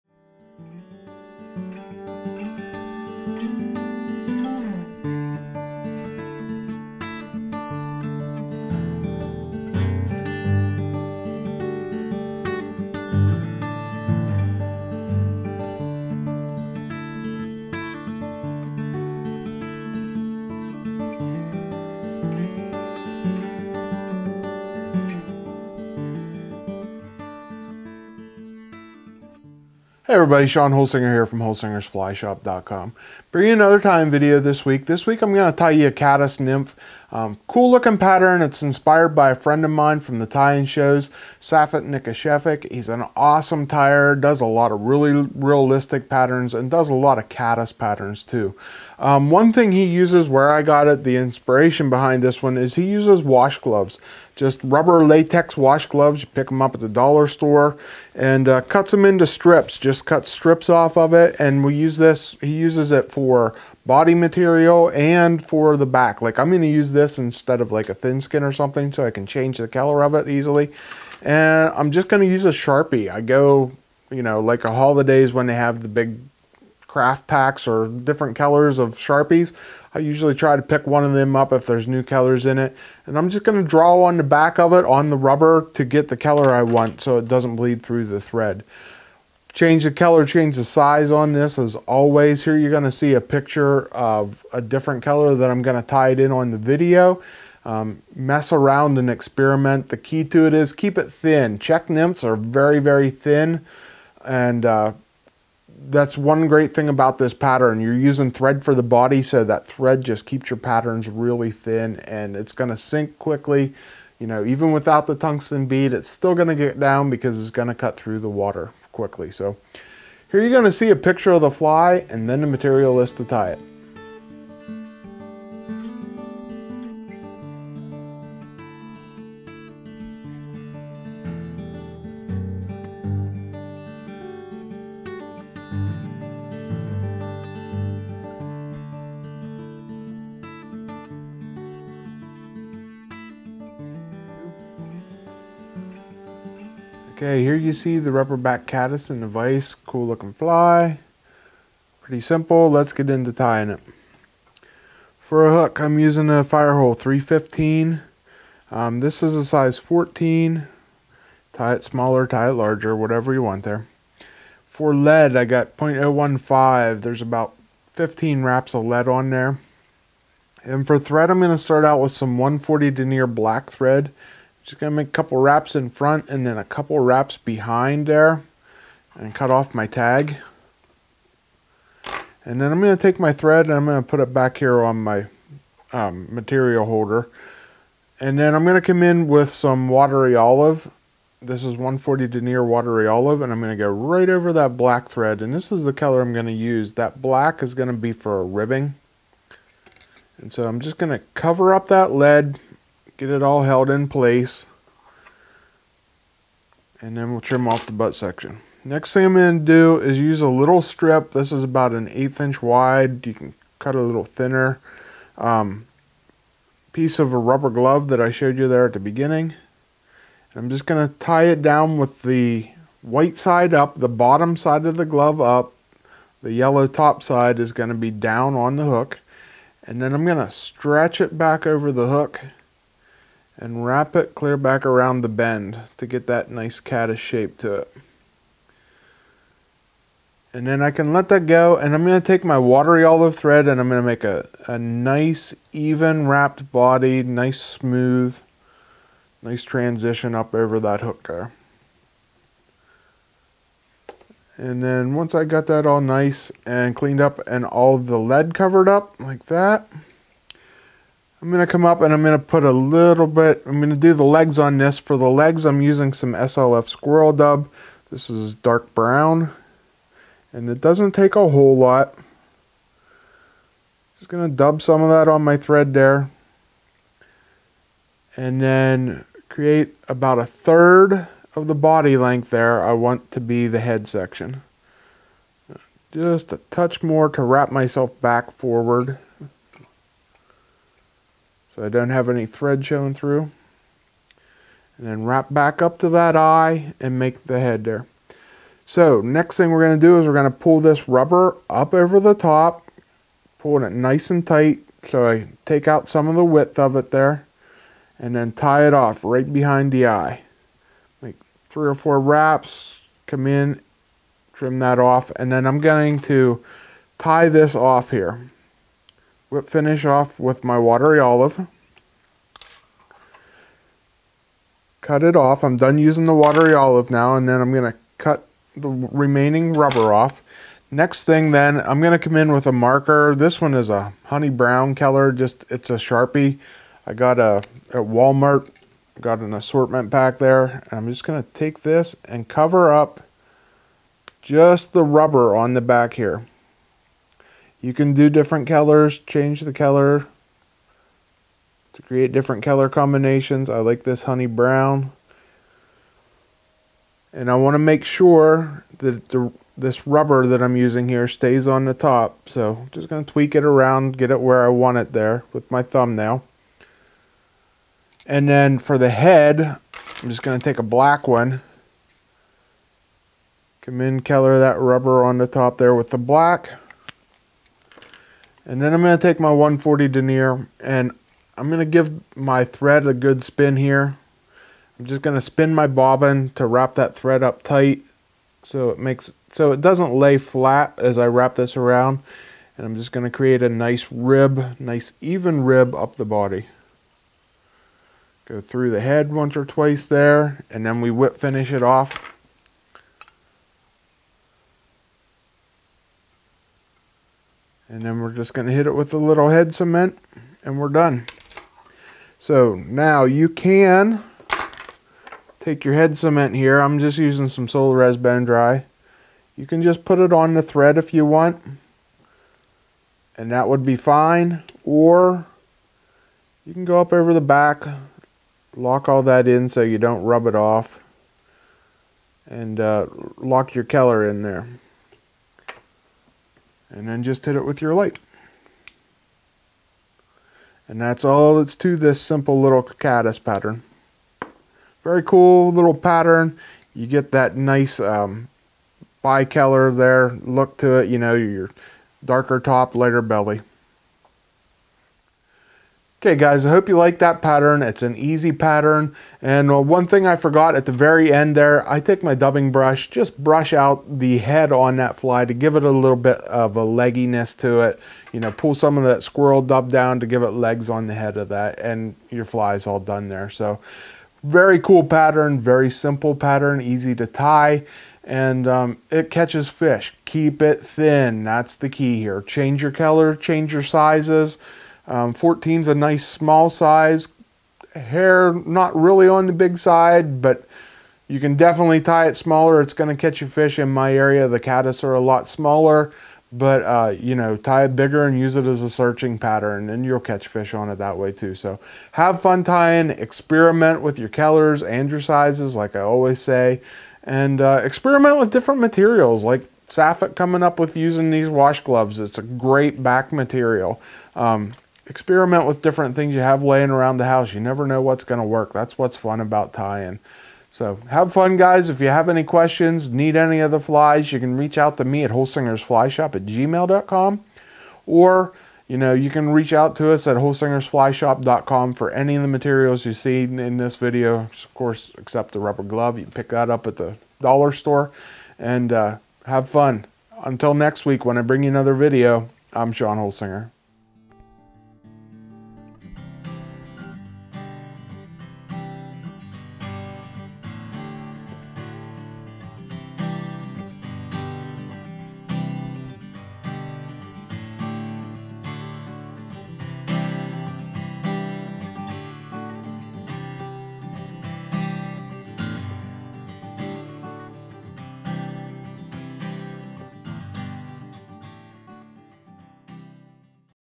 Rubber Backed Caddis, Holsinger's Fly Shop, Episode 495
This weeks video is one that is inspired by a tying friend of mine from the Fly Tying Show and Symposium, Safet Nikocevic.  Safet is an awesome tier and loves doing caddis patterns and can get pretty imitative with some of his flies.  Another thing that Safet is good at is using materials others might not think of like a latex rubber wash glove.  He has some really cool patterns using them and can change them to a different color with the use of a marker.  If you attend the shows on the east coast you should definitely check out his table if he is there.

This pattern is a pretty simple one with only a few materials which you probably already have, however is there for you for any of the materials you might need.  Thanks for always watching and supporting our channel and shop, we really appreciate it.
-Shawn

Materials List:

Firehole 315 Size 14
.015 Lead Wire
140 Denier UTC Thread Black
140 Denier UTC Thread Watery Olive
Rubber Glove
SLF Squirrel Dub Dark Brown
Sharpie Marker Brown and Black
Solarez Bone Dry